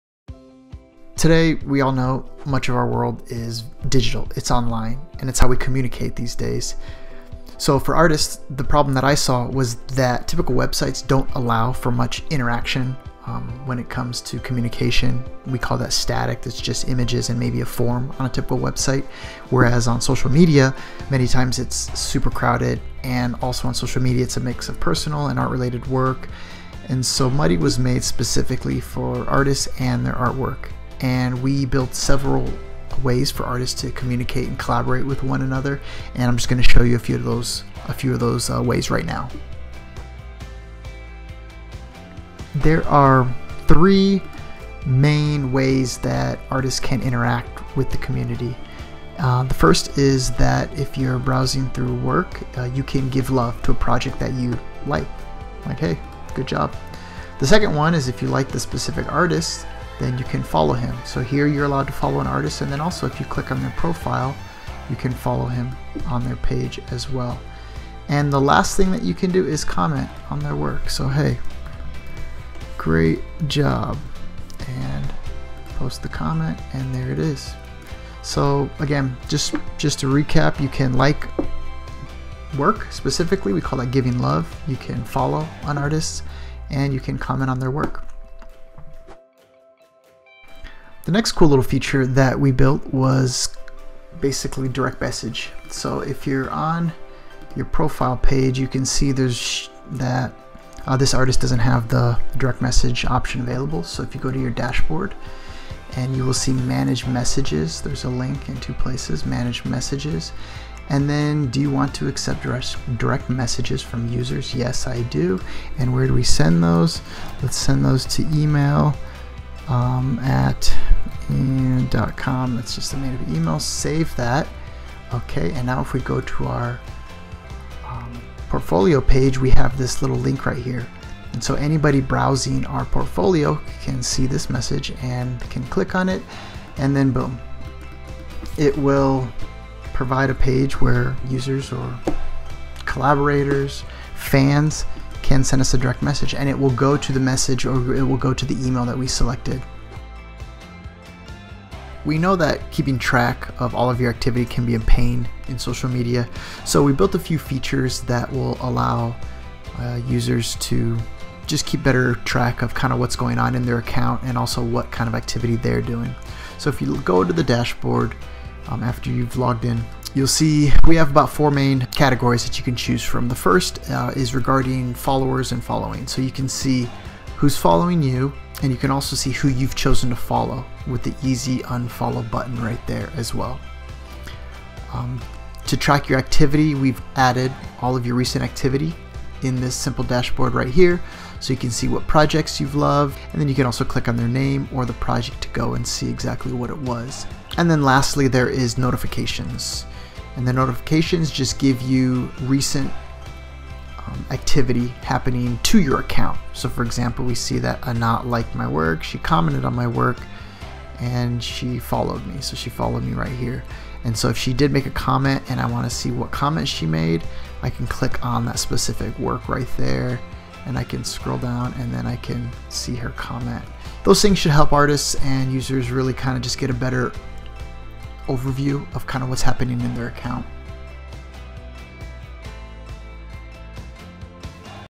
Mudddy Helps Artists Connect with the Community
Mudddy has been created for artists and enthusiasts that want to create, share, and connect.

To make it easier to connect, we added the same interactive social features that we’re all familiar with like the ability to comment, follow, and like.

These interactions are recorded in each dashboard, making it easy for artists to get a snapshot of their recent portfolio activity.